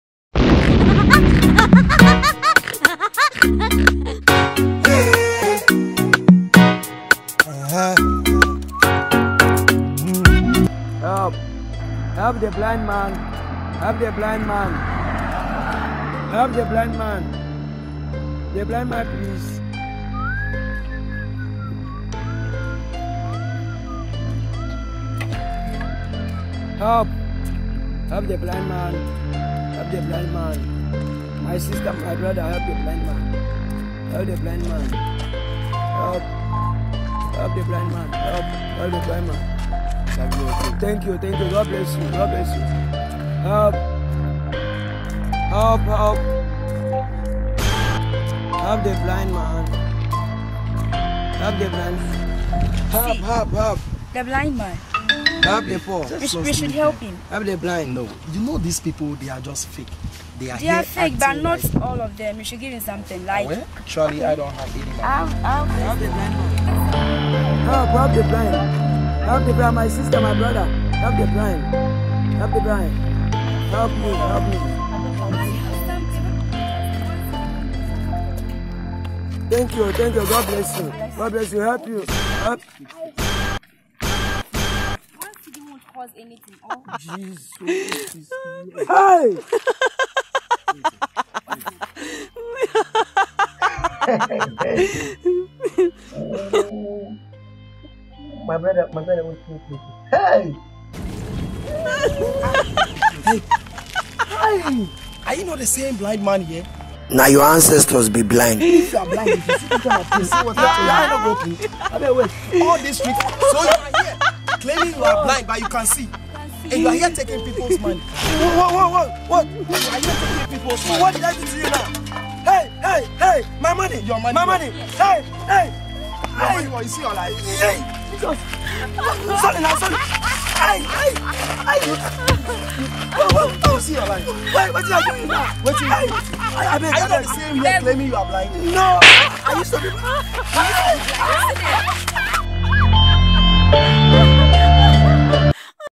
THE BLIND MAN (Funny boys Entertainment) EPISODE 12
This will be the most funniest video you will see this month, A blind man who suddenly gain his sight all because he saw a big bombom, watch this comedy and share, comment and give us a thumbs.